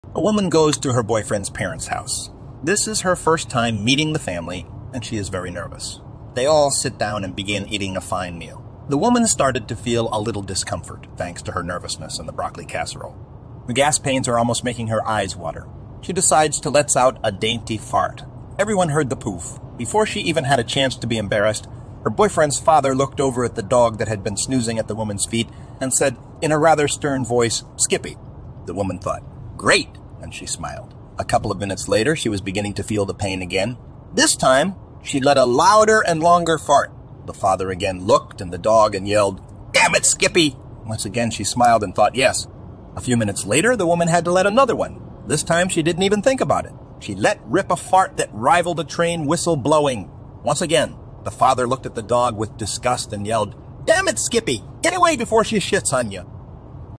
The first time at her boyfriend's parents' house - FUNNY JOKE 🤣
The first time at her boyfriend's parent house - FUNNY JOKE 🤣

The Joke 🤣👇:
A woman goes to her boyfriend's parents' house . This is her first time meeting the family and she is very nervous. They all sit down and begin eating a fine meal. The woman started to feel a little discomfort, thanks to her nervousness and the broccoli casserole. The gas pains are almost making her eyes water. she decides to lets out a dainty fart . everyone heard the pouf.  Before she even had a chance to be embarrassed, her boyfriend's father looked over at the dog that had been snoozing at the woman's feet and said in a rather stern voice, "Skippy!". The woman thought, "great!" and she smiled . A couple of minutes later, she was beginning to feel the pain again. This time, She let a louder and longer fart . The father again looked and the dog and yelled: "DAMMIT SKIPPY!!" Once again she smiled and thought "Yes!".  A few minutes later the woman had to let another one . This time she didn't even think about it. She let rip a fart that rivaled a train whistle blowing!  Once again, the father looked at the dog with disgust and yelled, "Dammit Skippy, get away before she shits on you!"